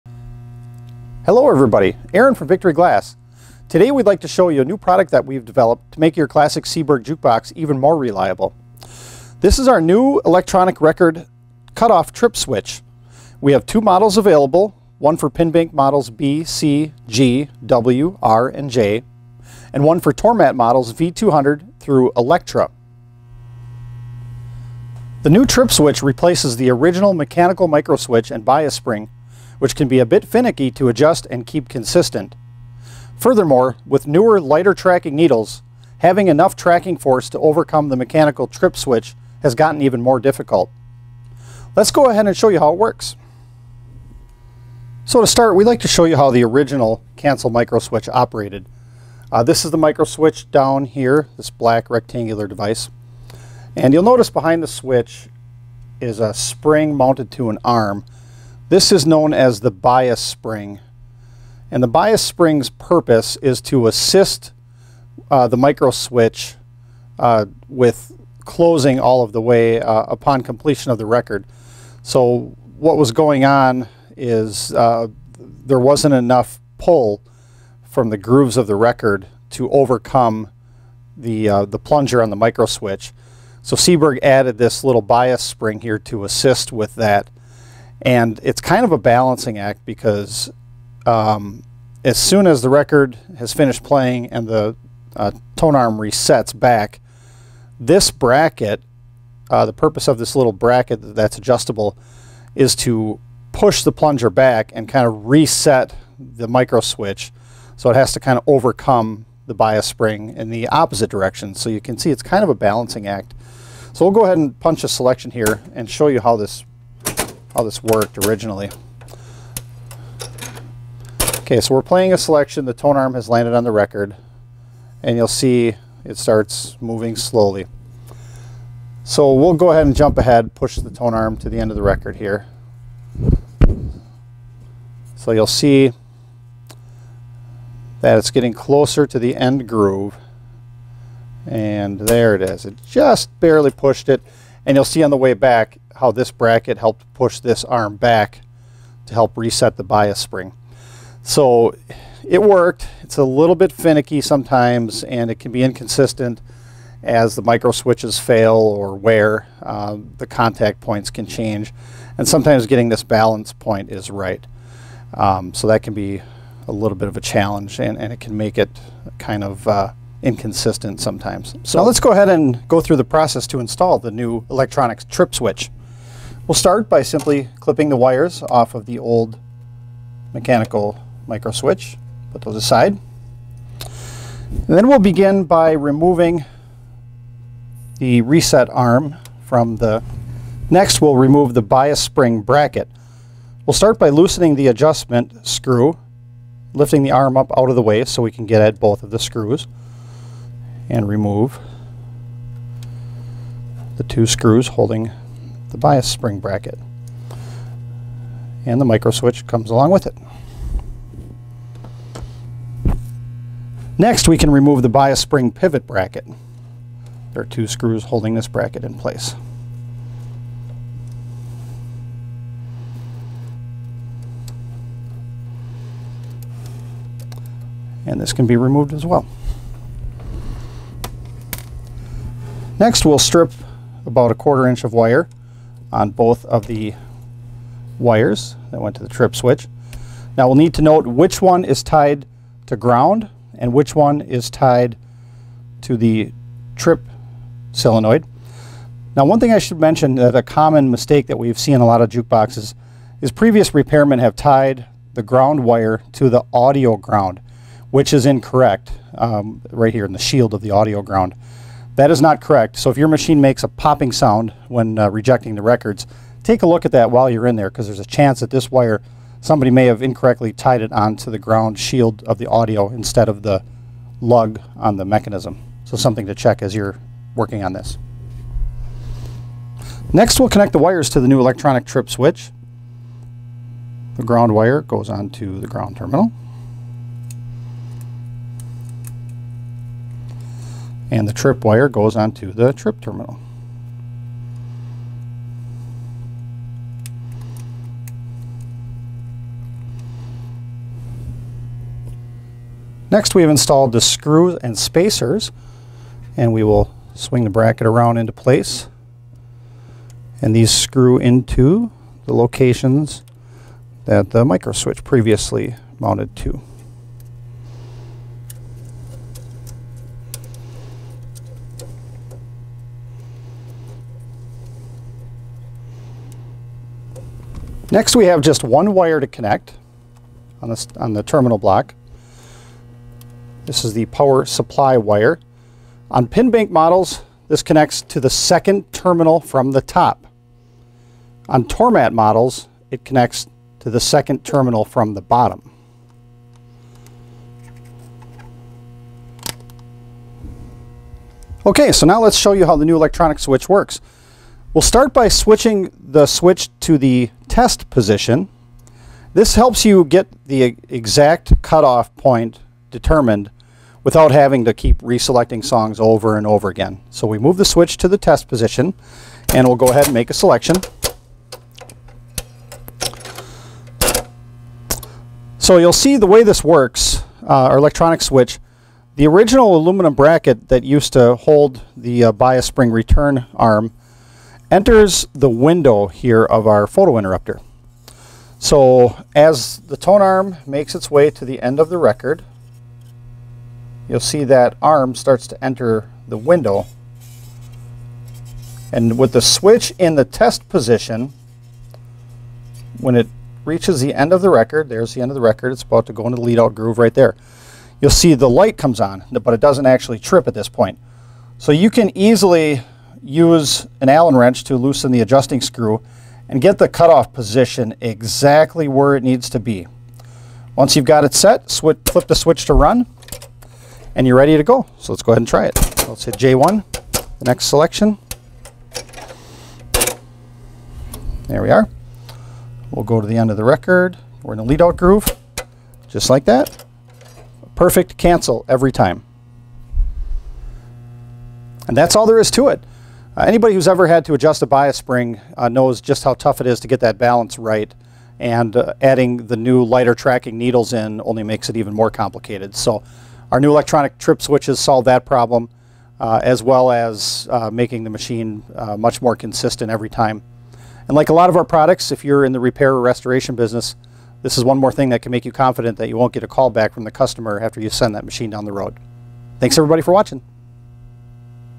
Introduction to the Victory Glass Electronic Trip Switch for Seeburg Jukeboxes
A Victory Glass exclusive! We have addressed another common trouble point on Seeburg jukeboxes - the mechanical cut-off trip microswitch and bias spring. Our new electronic board uses a photo interrupter "eye", eliminating the microswitch and bias spring.